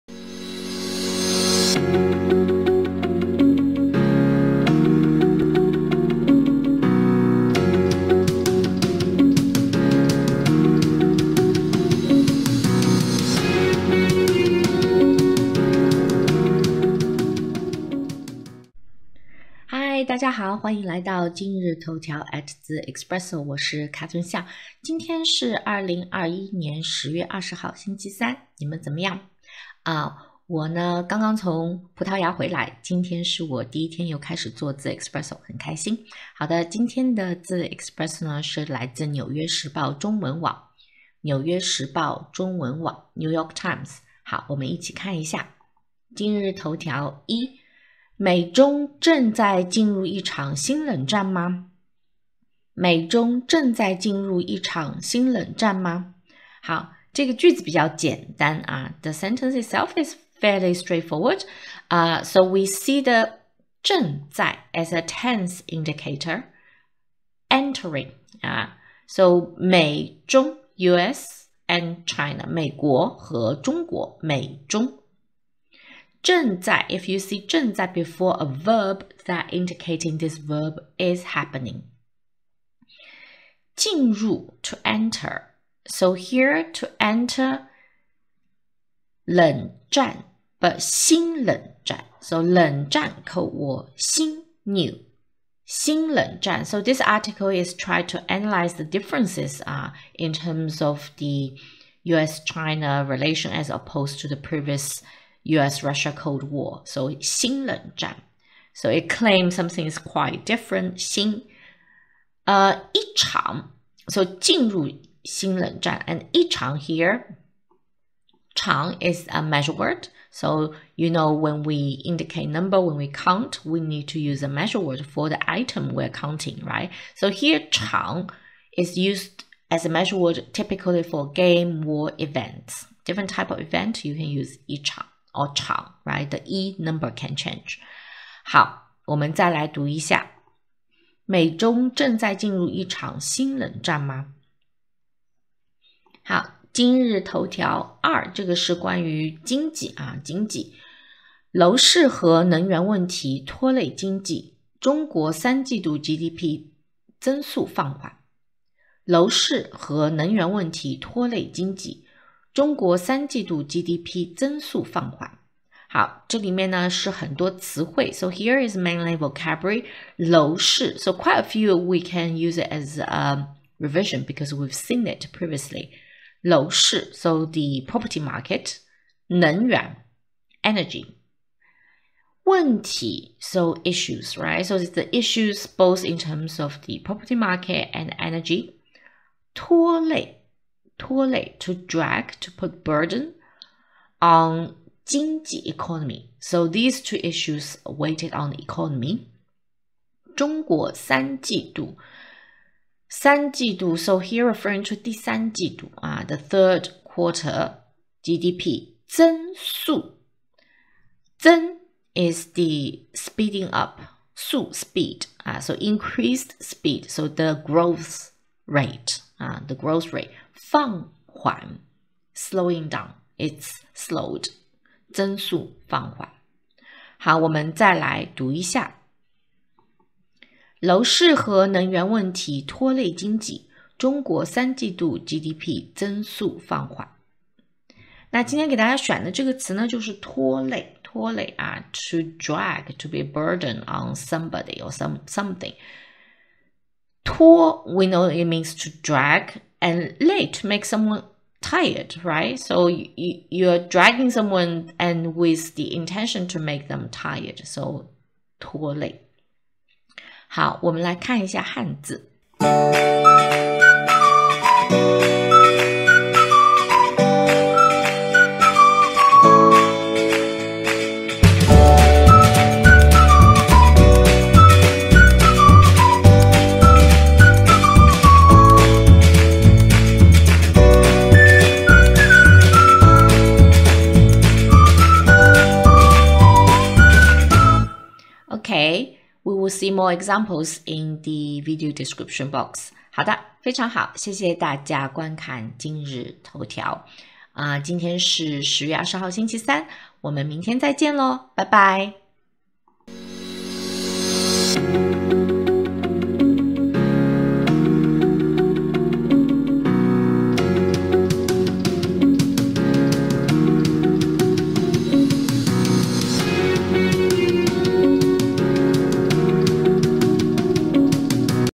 Learn Chinese from Daily Headline 今日头条 (Wednesday, 20 Oct 2021) - HSK 6
Welcome to Daily Headline 今日头条 at 字 espresso！

Today is Wednesday, 20 Oct 2021. Selected deadlines for you:

- The New York Times 中文网：美中正在进入一场新冷战吗？
- The New York Times 中文网：楼市和能源问题拖累经济，中国三季度GDP增速放缓

1. 进入 to enter
2. 一场 a (game, war, event) measure word
3. 冷战 cold war
4. 楼市  property market
5. 能源 energy
6. 拖累 to drag, to be a burden on
7. 放缓  to slow

* Key characters:
 拖累 to drag, to be a burden on (HSK 6)

Example Sentences：
1. 即使是一个错误都可以拖累你的生活，你可以想象一下坏习惯能带来多大的危害。
If even one mistake can drag your life down, you can imagine what kind of damage bad habits can do.
2. 更重要的是，我有了与以前使用电脑完全不同的经验，我觉得是在“控制”我的电脑，代替了以前的被它拖累，这种感觉真的很好。
I felt as though I was able to “control” my computer, for the firsttime, instead of being dragged by it. This feeling is pretty good.
3. 这些小的加总使得退却的消费者和公司没有进一步拖累经济。
These small pluses meant the retreat by consumers and firms did not drag the economy down by more.
4. 没有过去的拖累，一身轻松地全新开始，这是非常激动人心的；但是不承认我们曾经的失败并在前面阳光灿烂的新道路上做出纠正则是非常危险的。
It's energizing to begin with a clean slate without any drag from the past, but it can be dangerous to not acknowledge where we stumbled and make corrections along the shiny new path ahead.
5. 子女过多是个拖累。
Too many children are a burden.

Follow more conversations and learn more Chinese:

- Purchase my textbook if you are a complete beginner